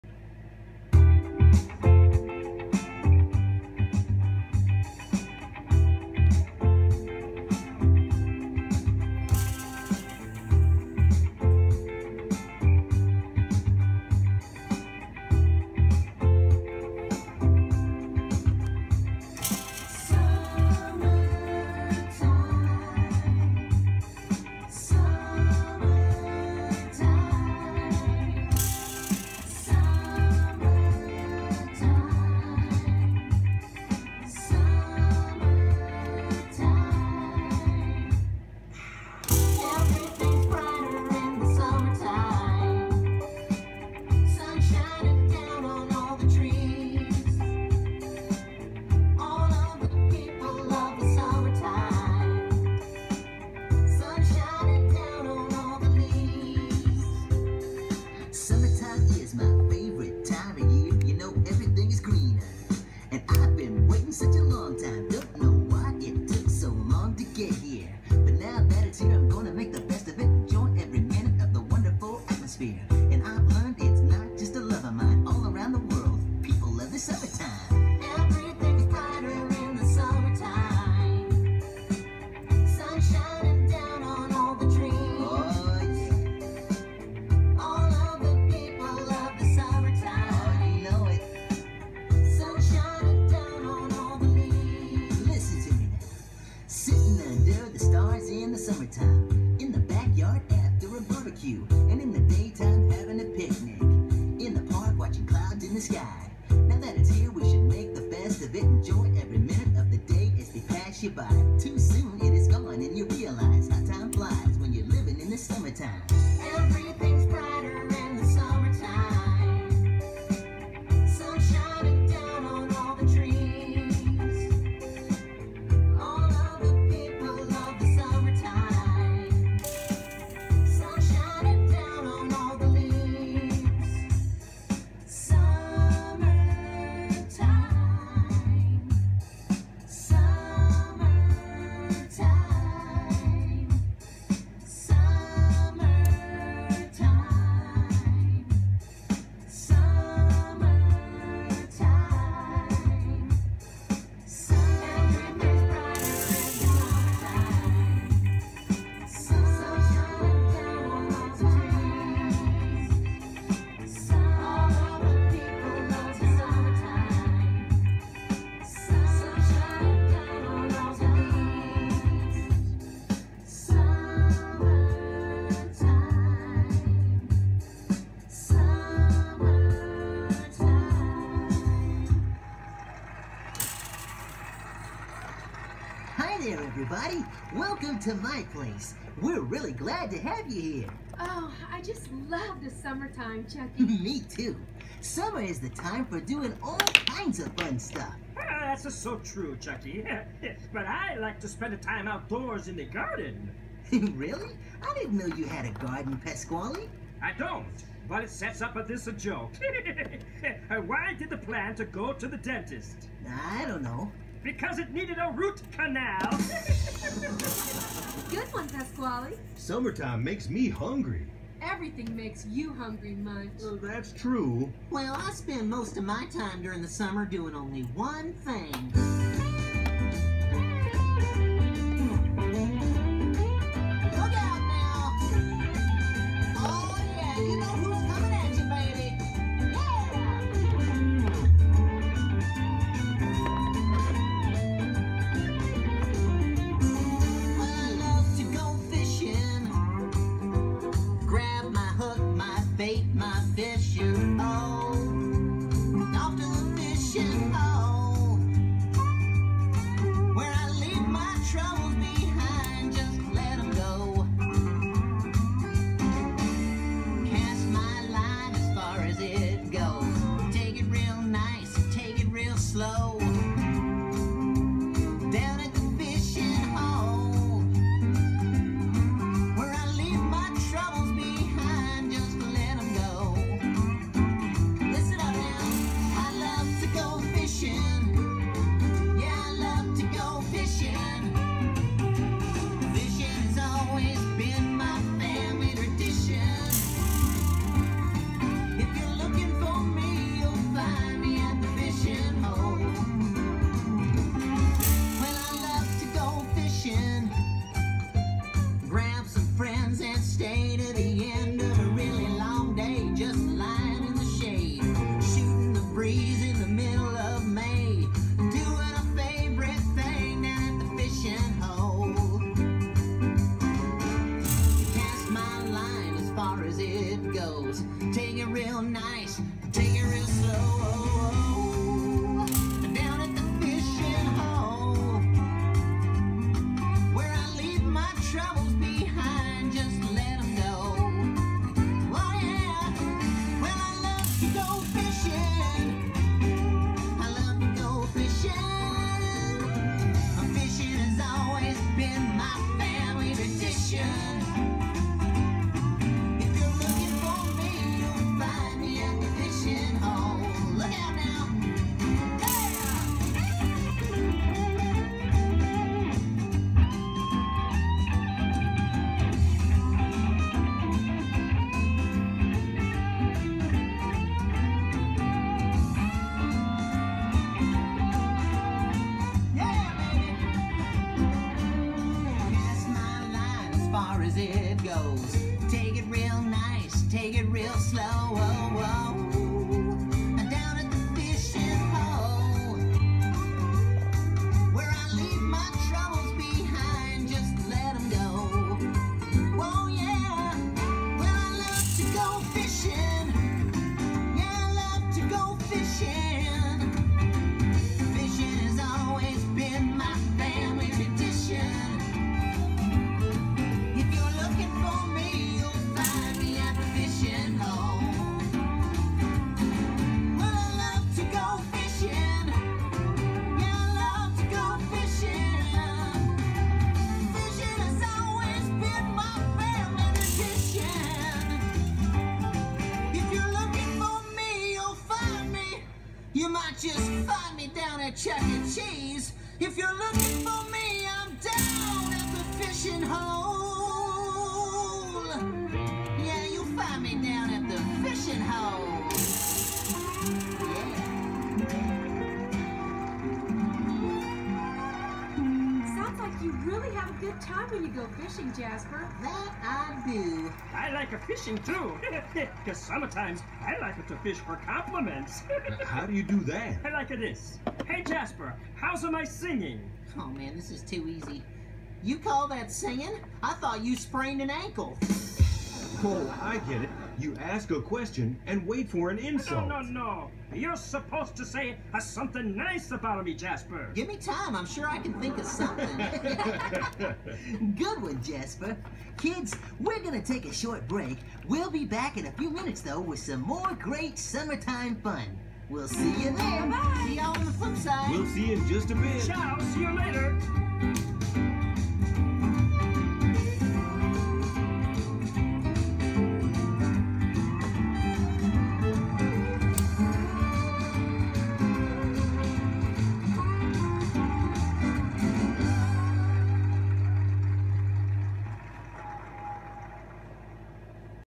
Chuck E Cheese Summer 2001-2006 segment 1
This is segment 1 of the Summer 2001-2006 show performed by my CEC figures. via YouTube Capture